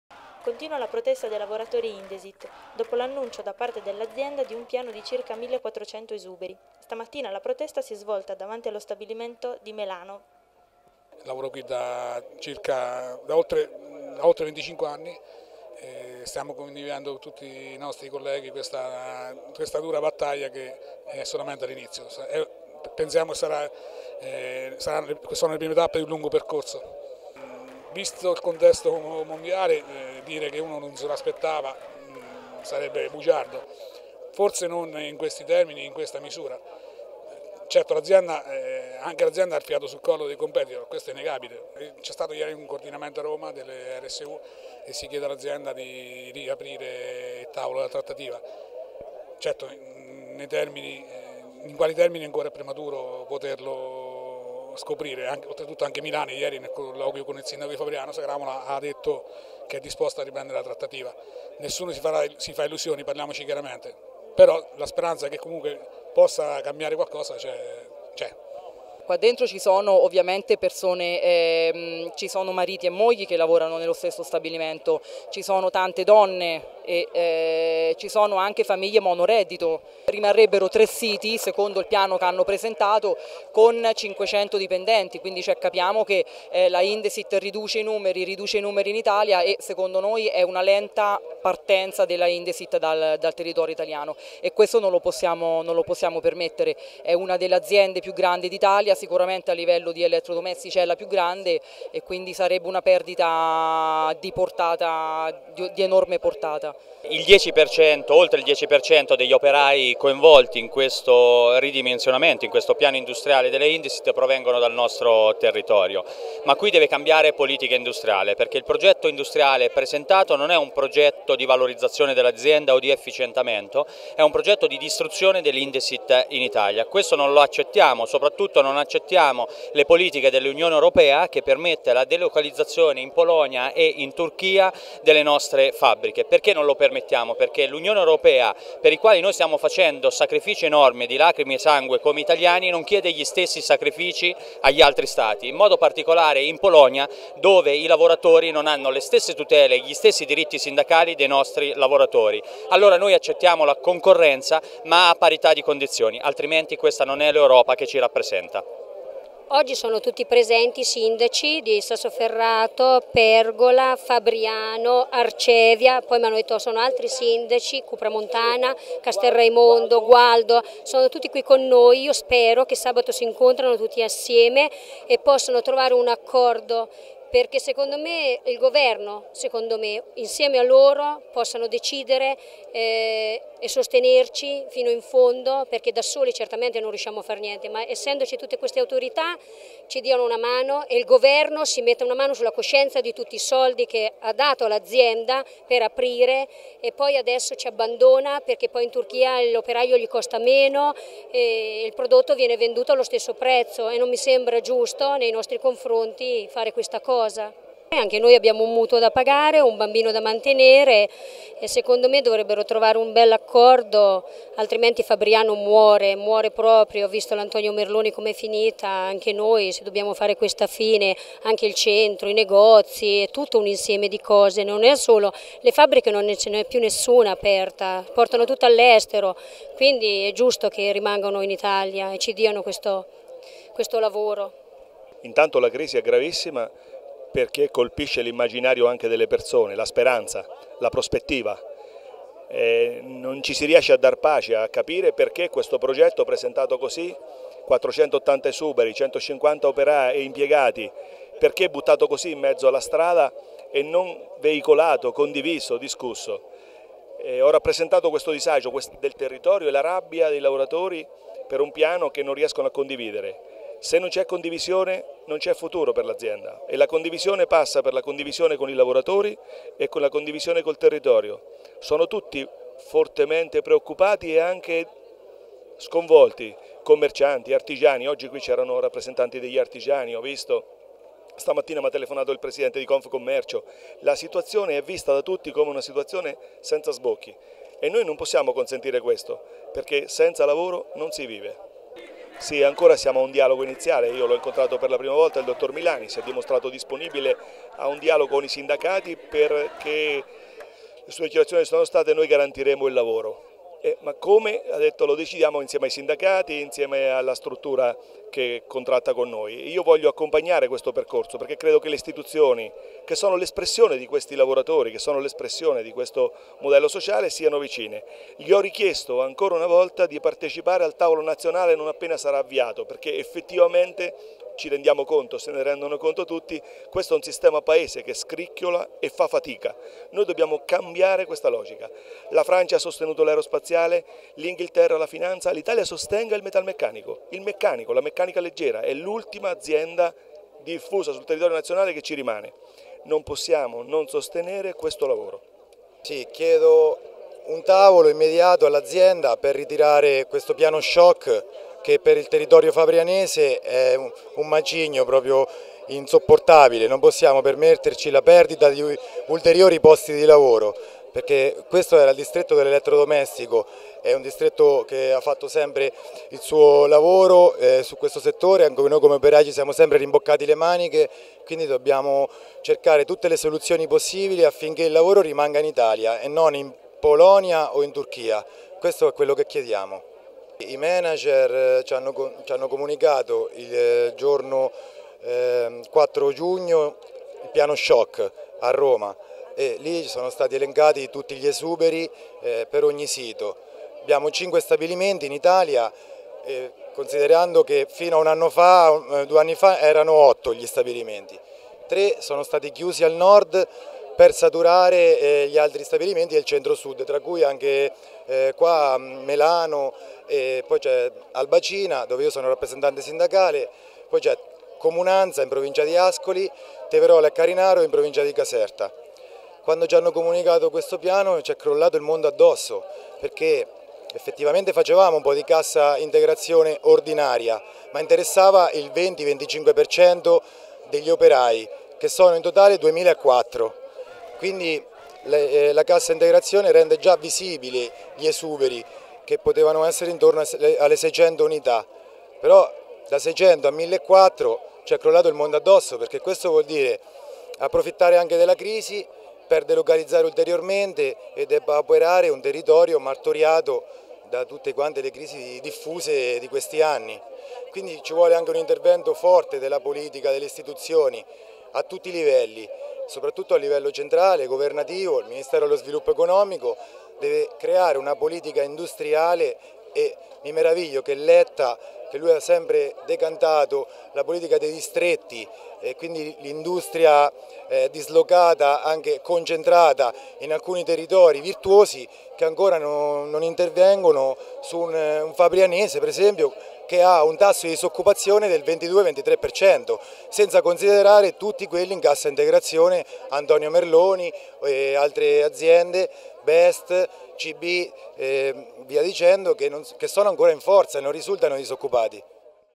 La voce della protesta dall'Indesit di Melano
Determinati e preoccupati i lavoratori degli stabilimenti Indesit di Albacina e Melano continuano la loro protesta per impedire che il piano di ristrutturazione annunciato a inizio giugno dall'azienda  venga attuato.
Ciò che più temono è che questo sia solo il primo passo di un graduale e completo abbandono dell'Italia da parte della società, privando il territorio della sua principale risorsa economica, ma soprattutto di un punto di riferimento fondamentale per l'identità di Fabriano e dei suoi abitanti.
Tra le persone che stamattina si sono riunite per protestare davanti allo stabilimento di Melano emerge forte non solo la preoccupazione per la perdita del proprio posto di lavoro, ma anche l'amarezza nei confronti di un piano aziendale che sembra non tener conto dei sacrifici, dell'impegno e della dedizione di quelle persone che, col proprio lavoro hanno contribuito alla crescita e allo sviluppo dell'azienda.

Riprese e montaggio: Domenico Nicoletta Puzzillo